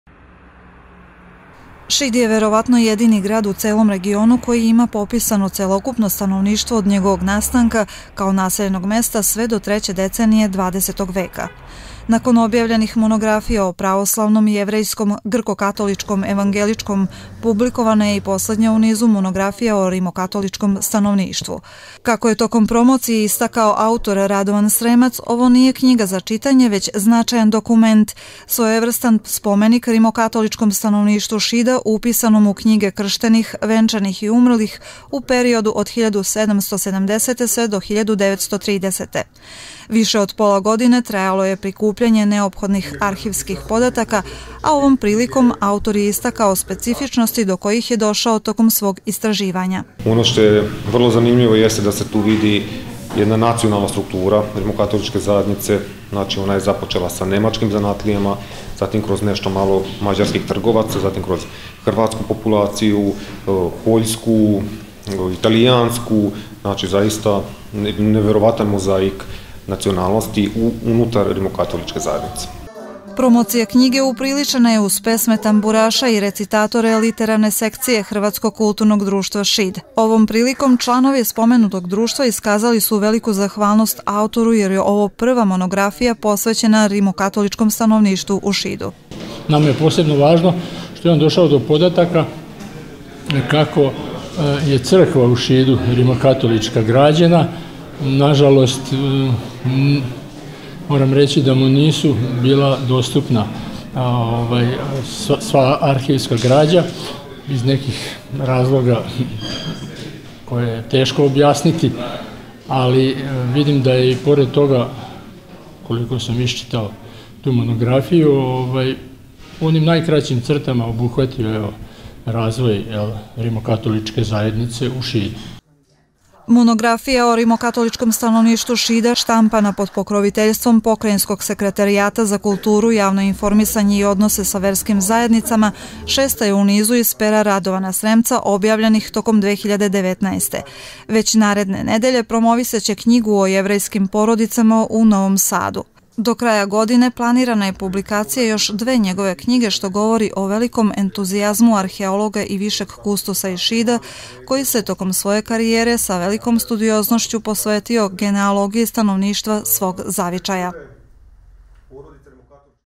Monografija o rimokatoličkom stanovništvu Šida
U sali Muzeja naivne umetnosti „Ilijanum“ u Šidu sinoć je predstavljena monografija  o rimokatoličkom stanovništvu Šida autora Radovana Sremca.Ovo je ujedno poslednja monografija u sklopu projekta   kojim je obuhvaćeno  celokupno stanovništvo ovog grada i pojedinih okolnih sela